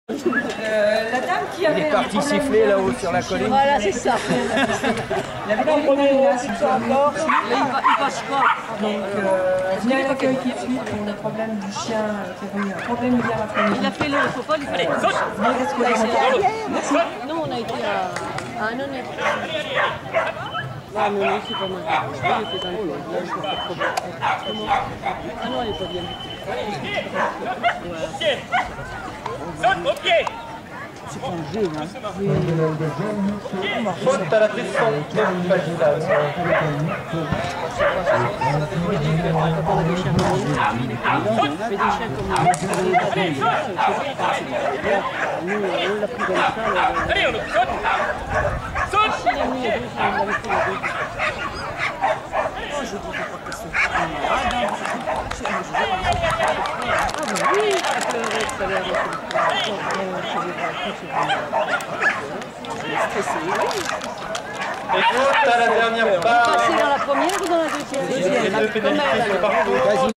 URLAUT Bouvier Allemand manche 1 agility cat D
Finale du Grand Prix de France d'agility de la SCC, Le Creusot, juillet 2010, manche 1 agility, catégorie D